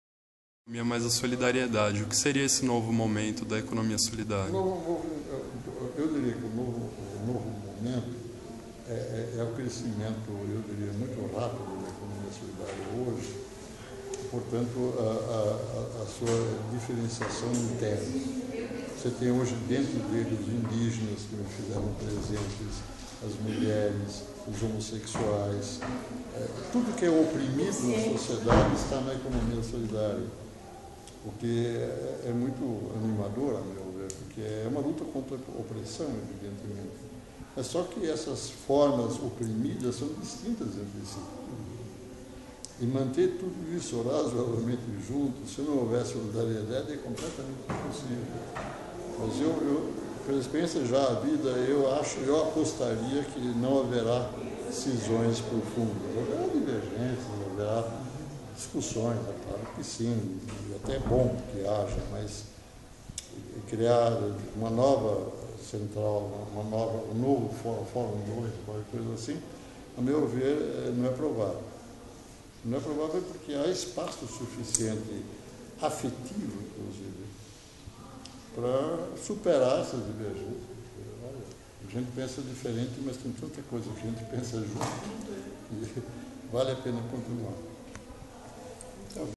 Entrevista Paul Singer e Ademar Bucci IV.mp4
Paul Singer e Ademar Bucci falam sobre economia solidária.